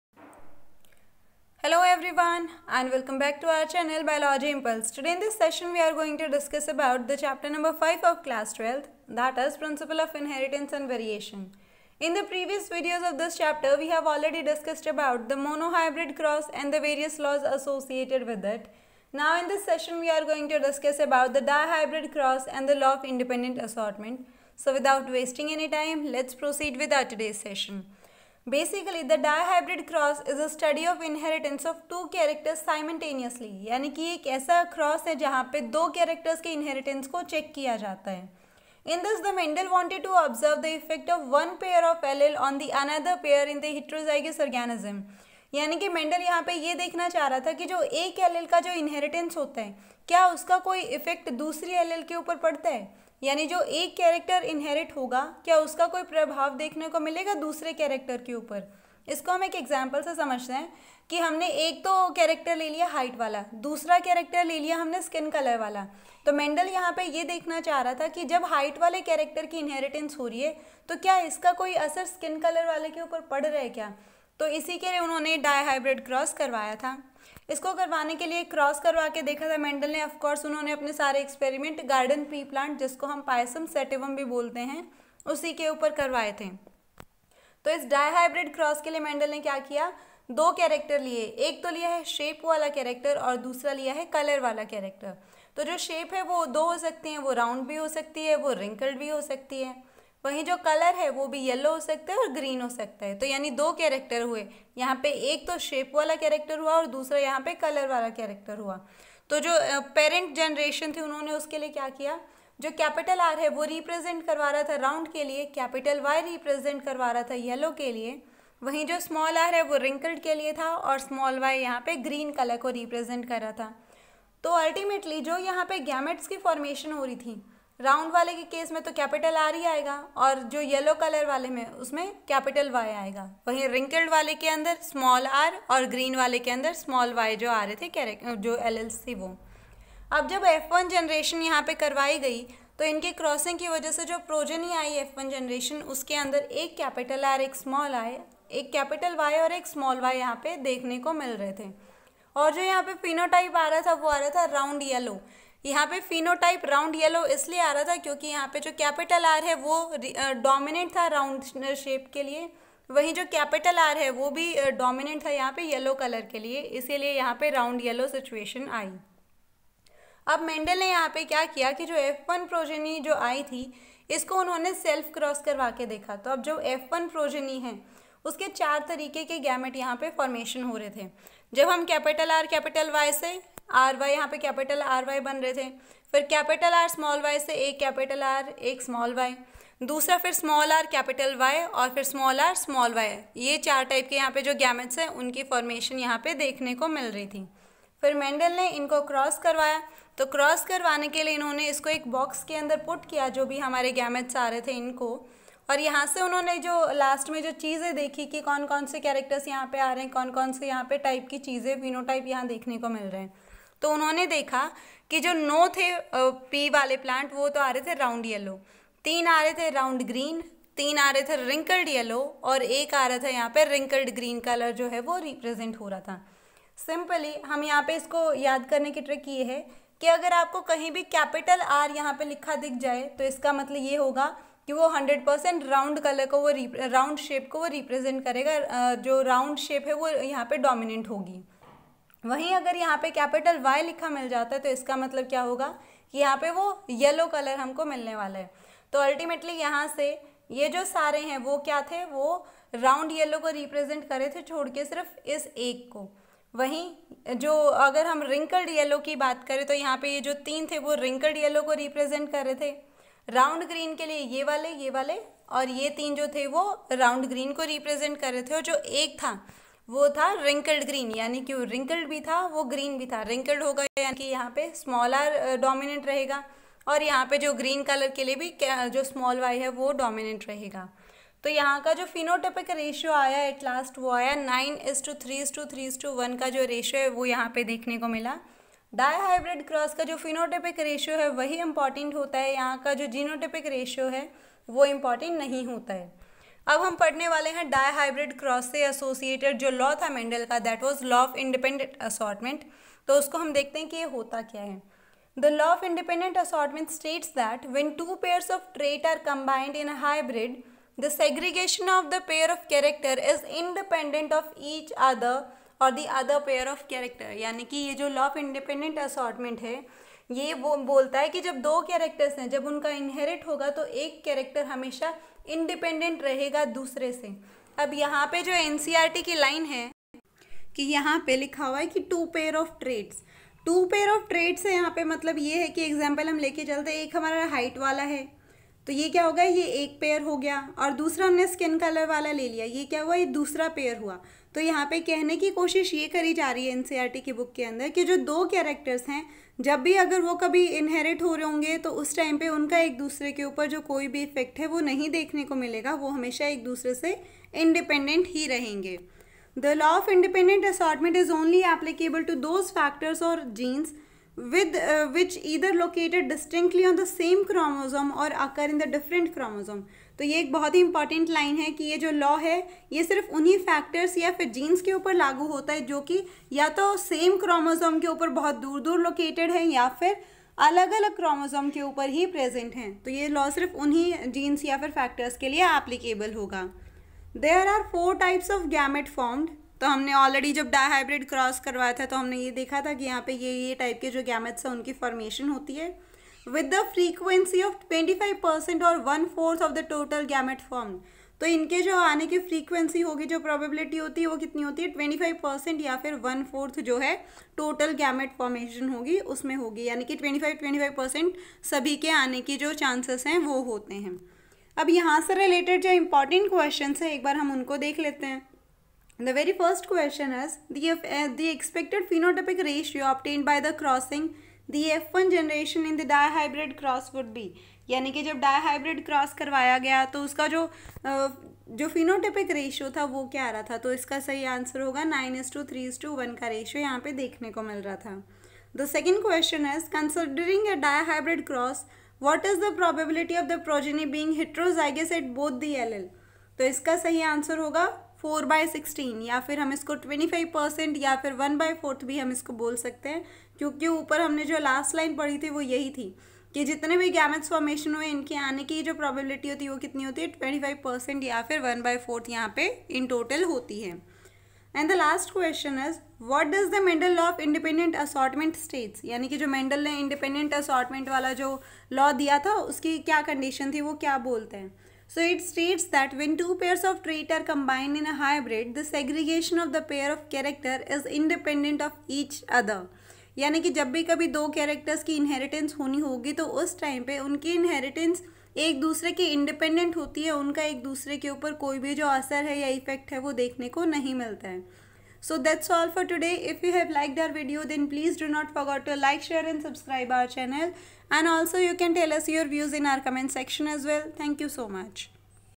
CLASS 12 | BIOLOGY | CHAPTER 5 | PRINCIPLE OF INHERITANCE AND VARIATION | HAND WRITTEN NOTES |PART 3
In this session we will discuss about dihybrid cross and law of independent assortment .